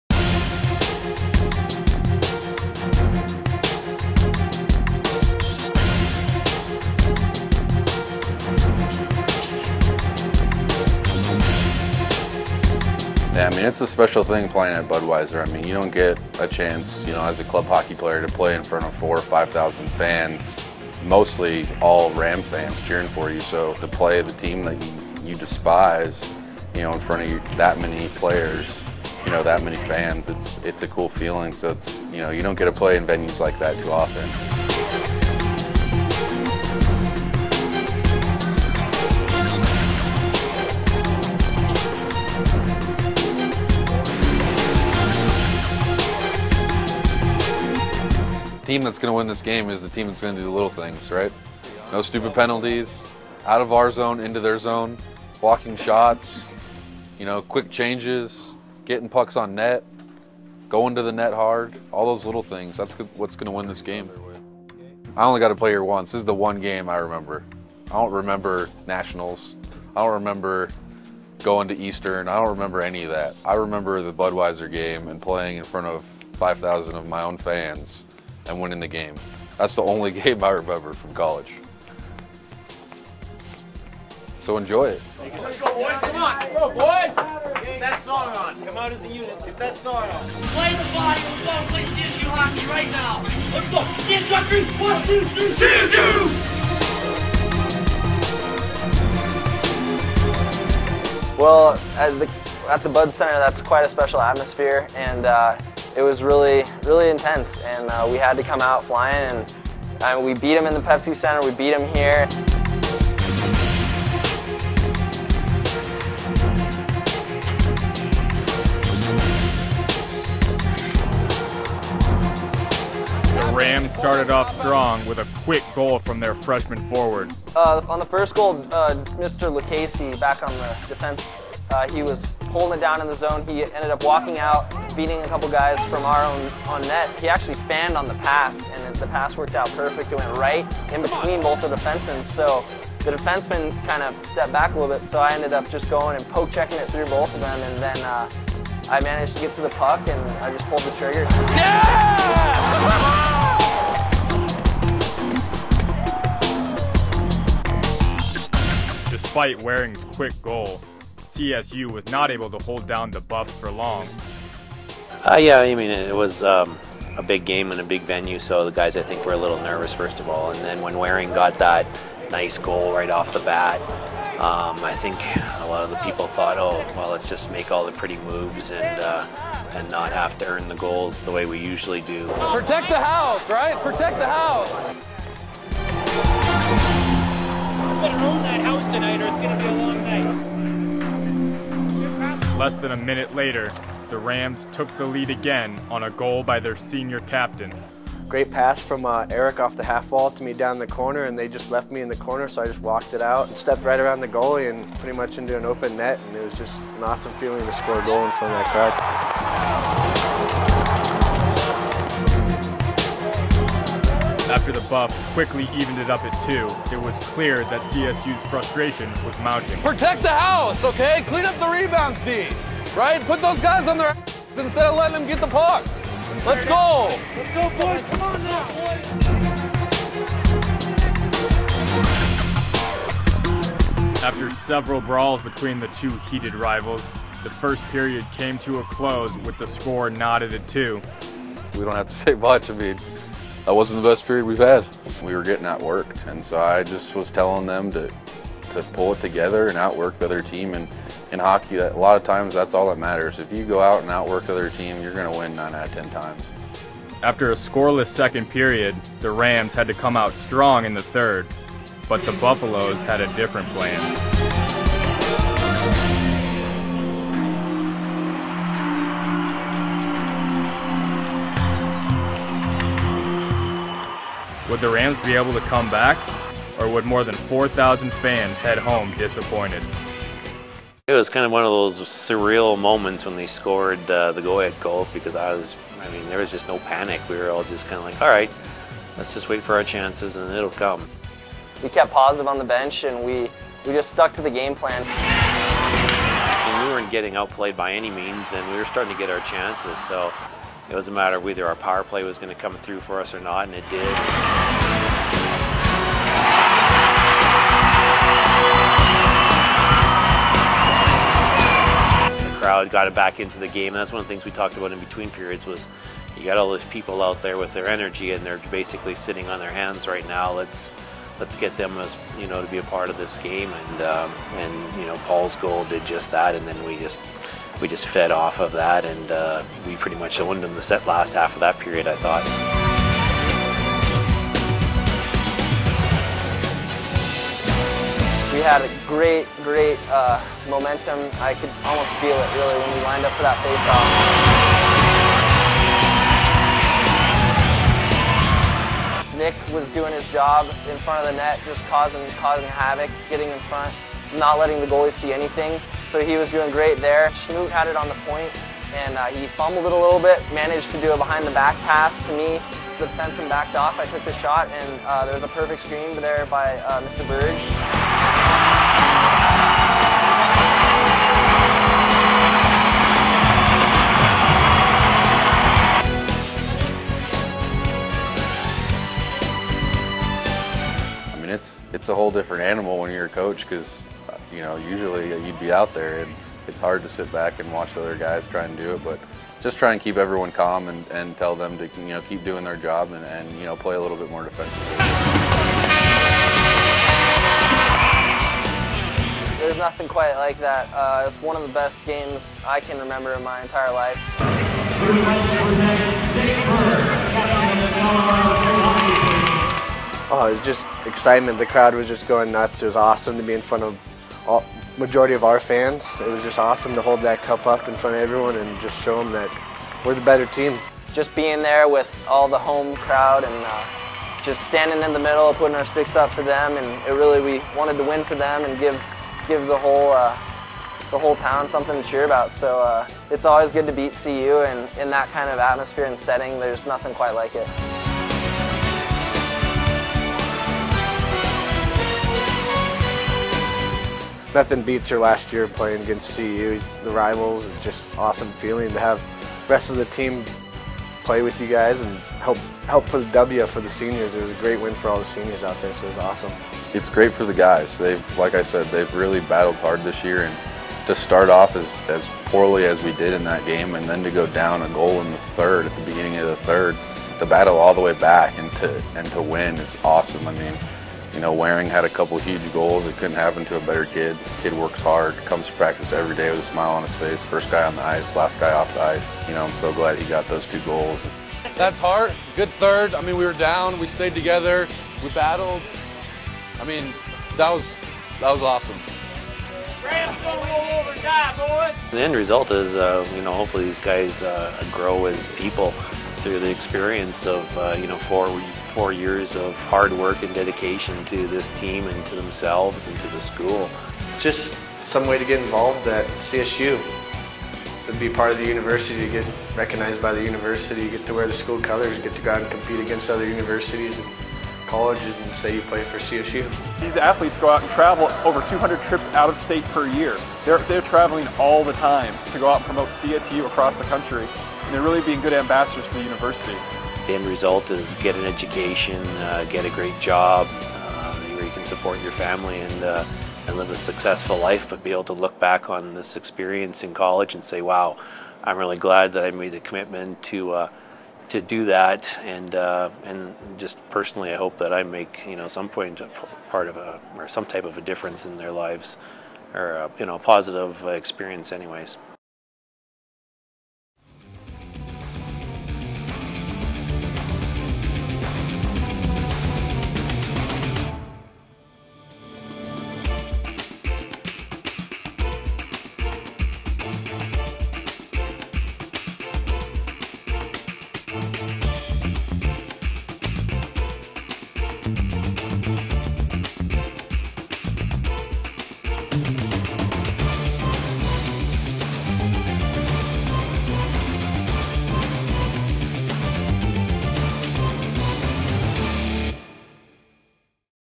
"Club Ram" Excerpt
A Colorado State Video Production which I created, shot, wrote, edited, narrated..everything.  Currently working on the second episode about the CSU Rugby Team.